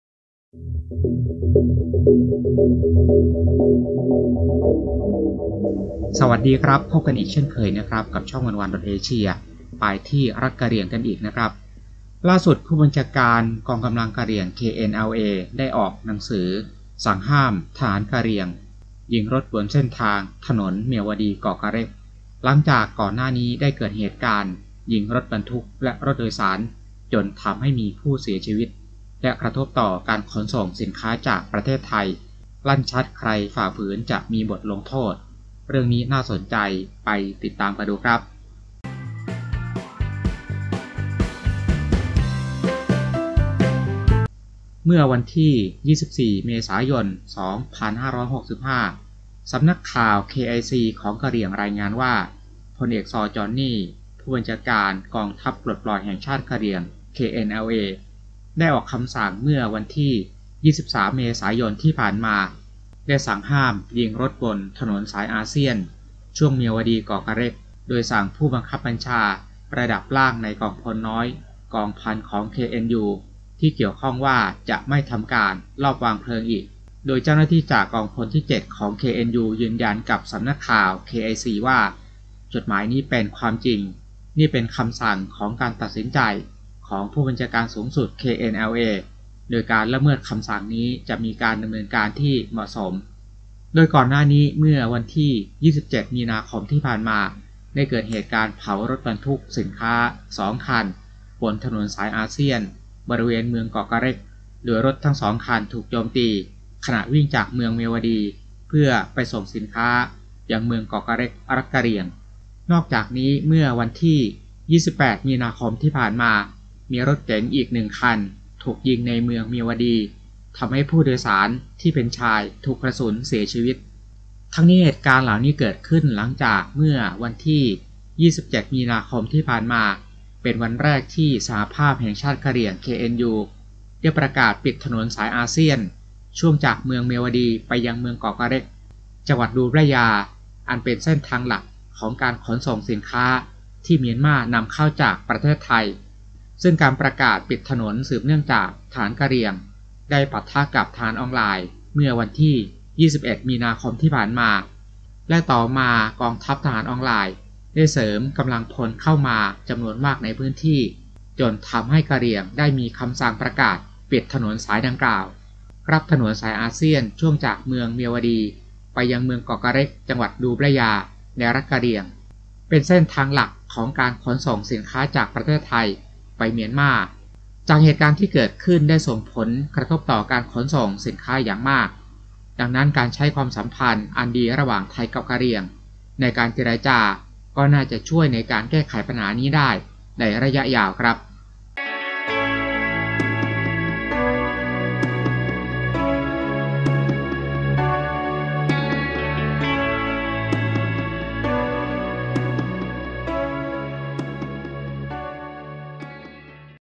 ผบ.กะเหรี่ยง KNLA สั่งห้ามยิงรถบรรทุก-รถพลเรือนในพื้นที่
ไปที่รัฐกะเหรี่ยงกันอีกครับ ล่าสุด ผู้บัญชาการกองกำลังกะเหรี่ยง KNLA ออกหนังสือสั่งห้ามทหารกะเหรี่ยง ยิงรถบนเส้นทางถนนเมียวดี-คอคาเรก หลังจากก่อนหน้านี้ได้เกิดเหตุการณ์ยิงรถบรรทุก และรถโดยสารจนทำให้มีผู้เสียชีวิต และกระทบต่อการขนส่งสินค้าจากไทย ลั่นชัดใครฝ่าฝืนจะมีบทลงโทษ เรื่องนี้น่าสนใจไปติดตามกันครับ คลิกอ่านรายละเอียด